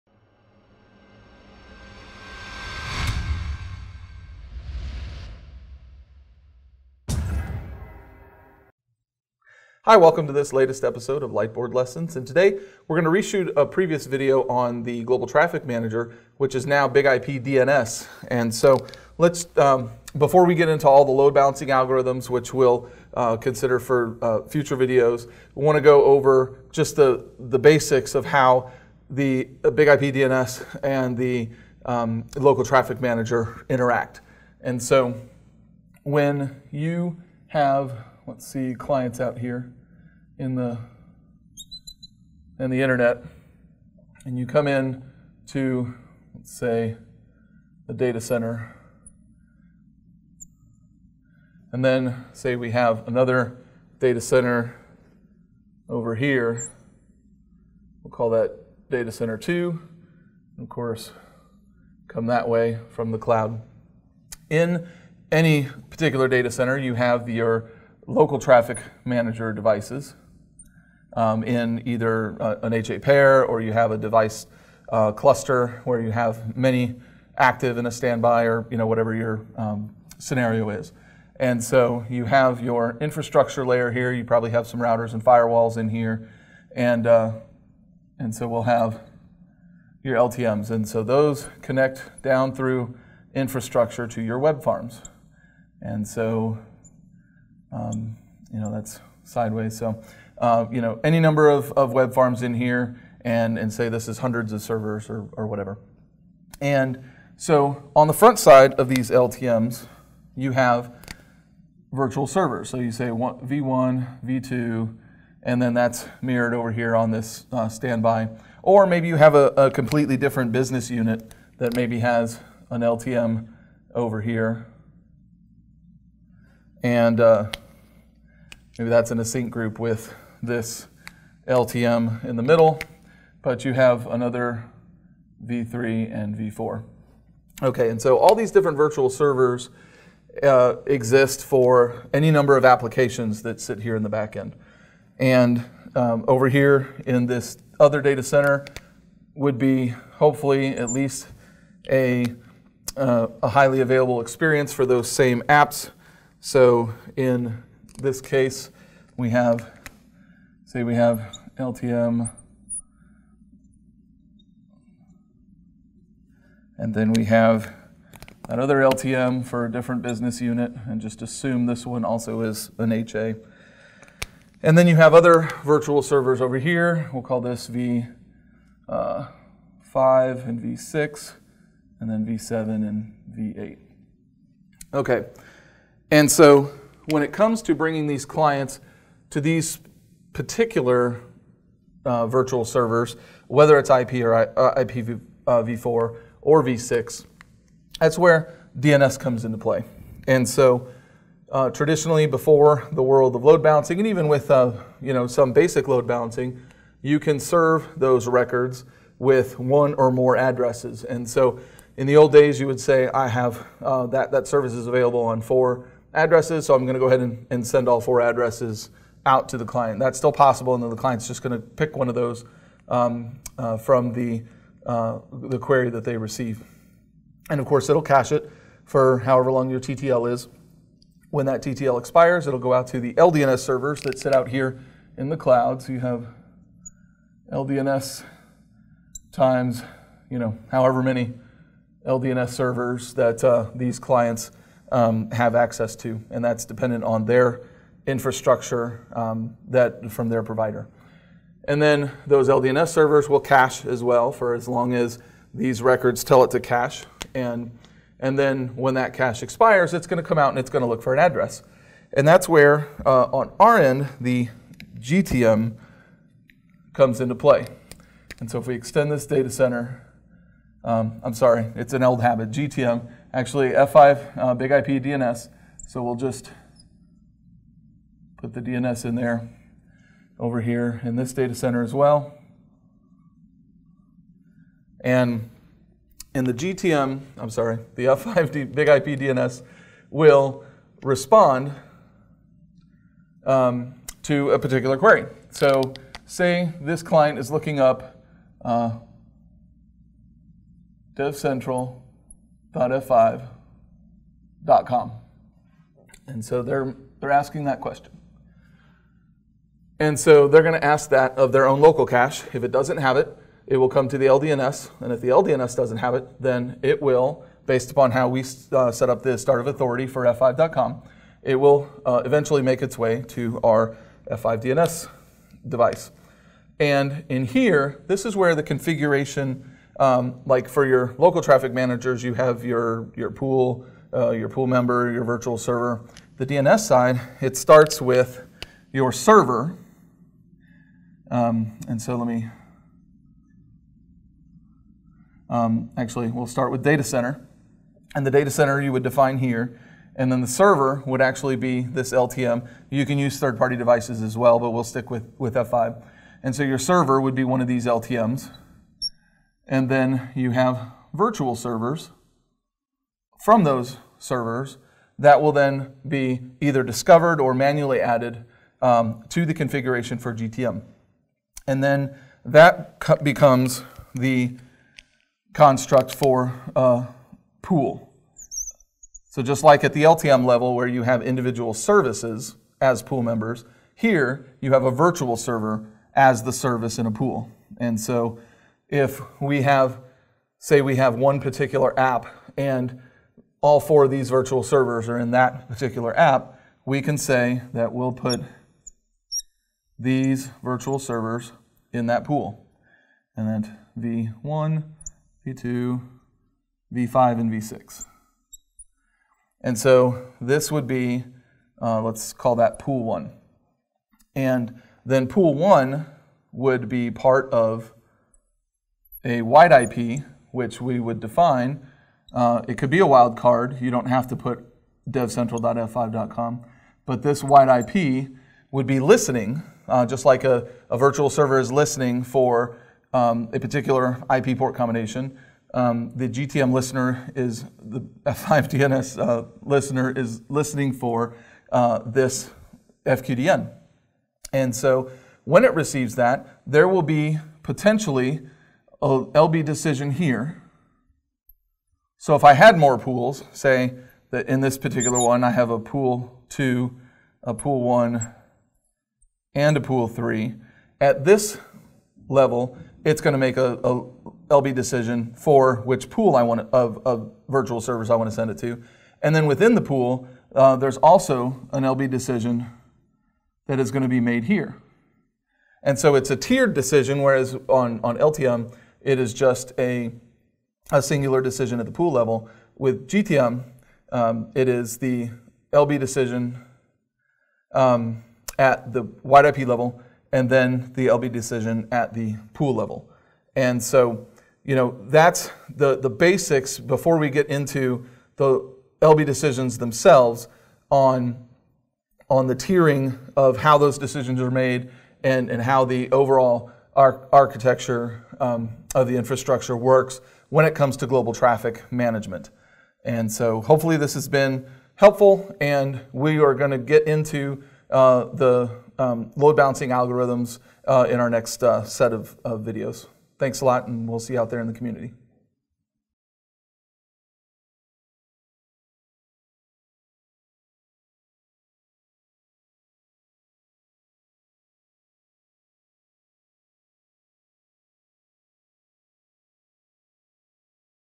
BIG-IP DNS Load Balancing Introduction
This video provides an introduction to BIG-IP DNS load balancing, focusing on its functionalities and algorithms. The session revisits the global traffic manager, now known as BIG-IP DNS, and outlines key concepts related to load balancing strategies. Viewers will gain insights into how BIG-IP DNS optimizes traffic management for improved performance and reliability.